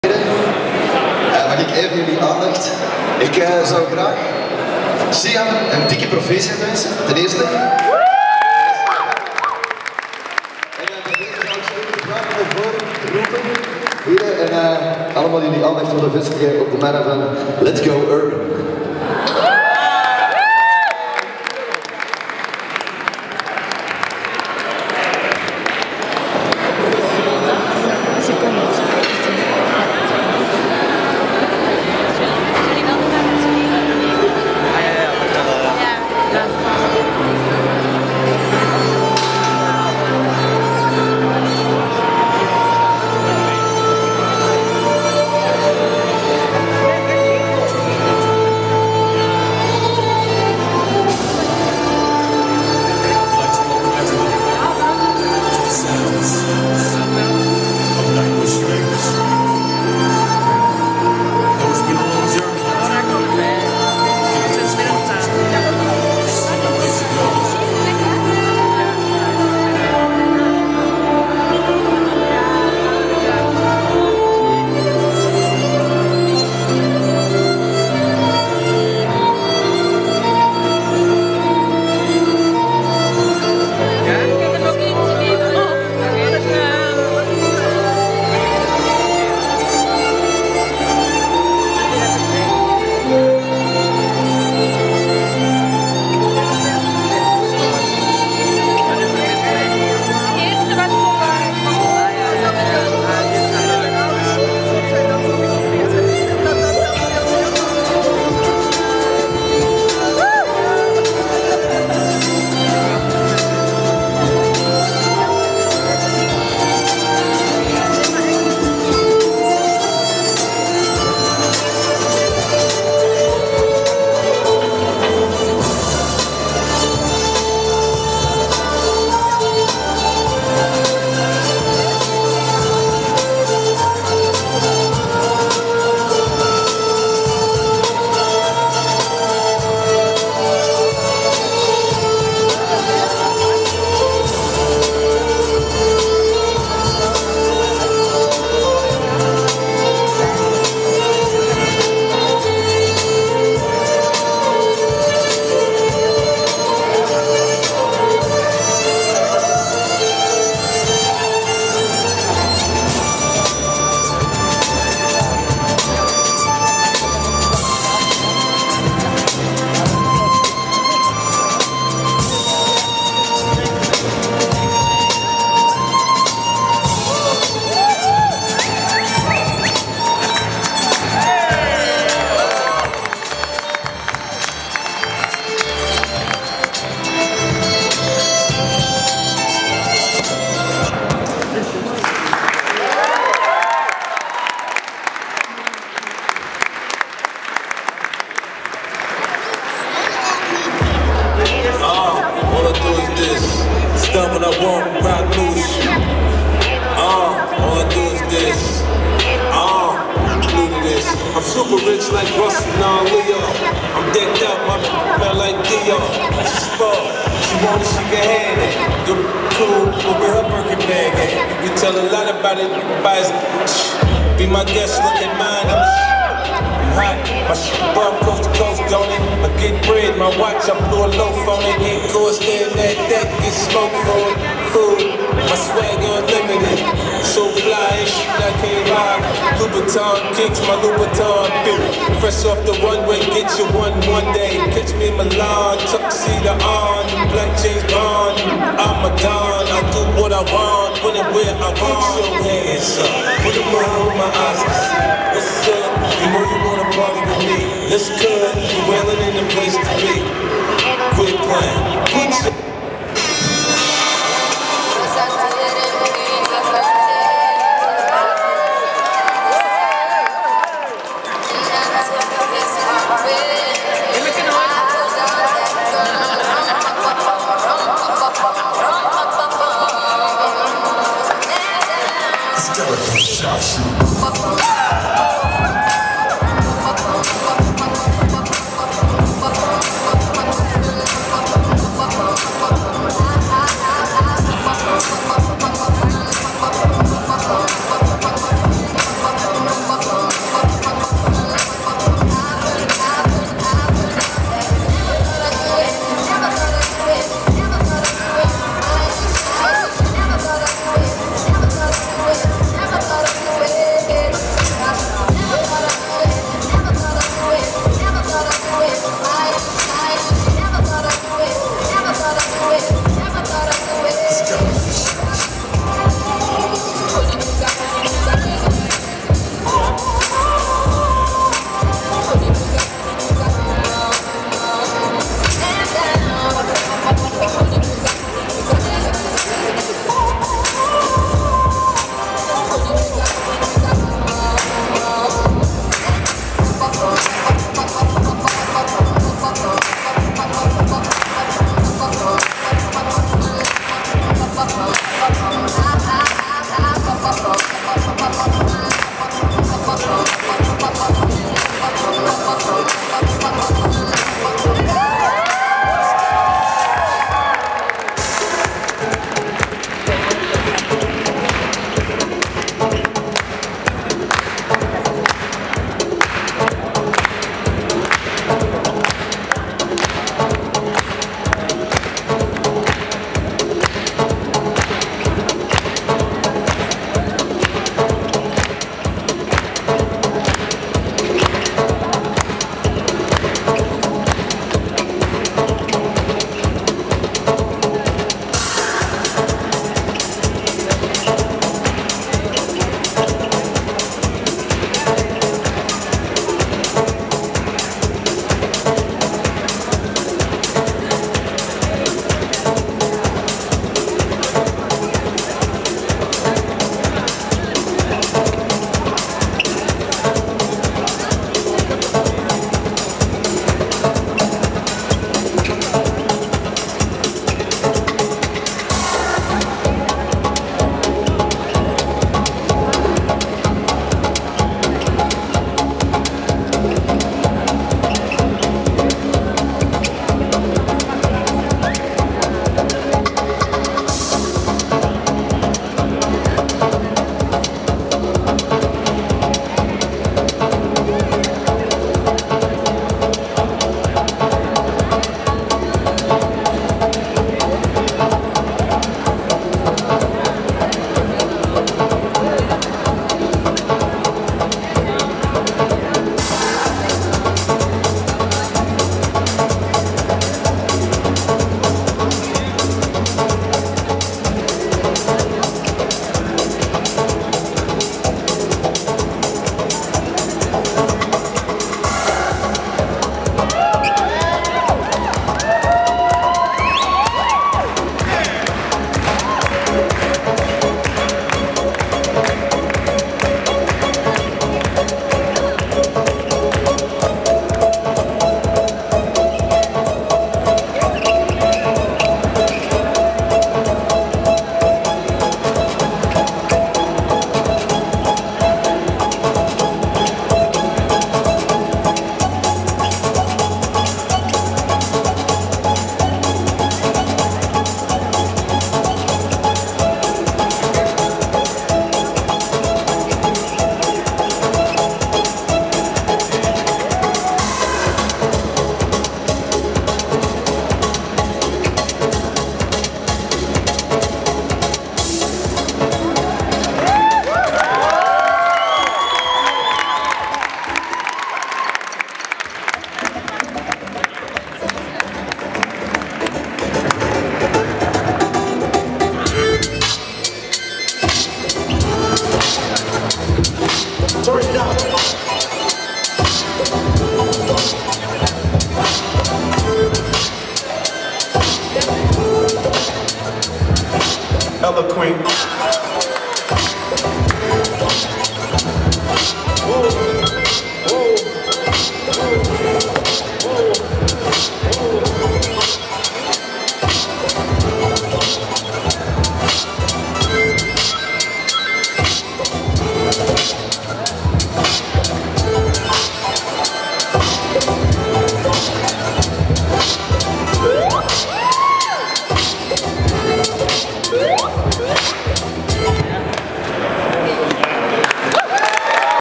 Let's Go Urban @ Antwerpenaar van het Jaar - Sihame El Kaouakibi
Spontane act tijdens de after party van de verkiezing "Antwerpenaar van het Jaar 2011" met als terechte winnares Sihame El Kaouakibi, oprichtster van Let's Go Urban !